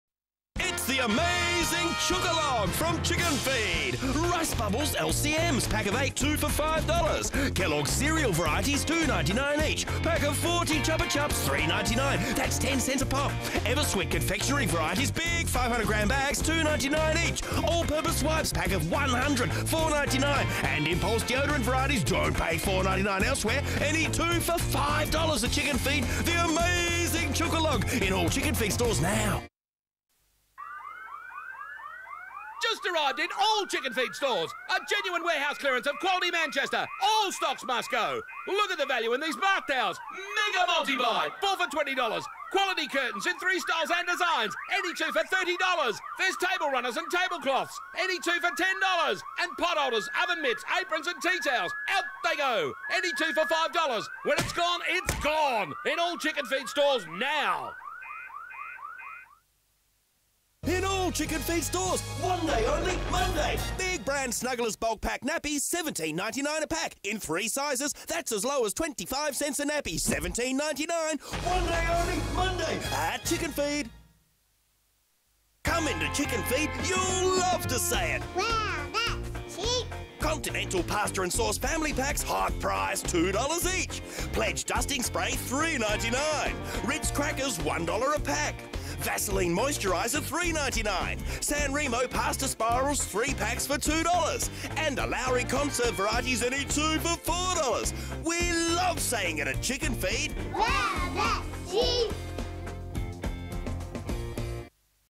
Chickenfeed Ads Vol 9
4 ads for the now closed Tasmanian discount retail stores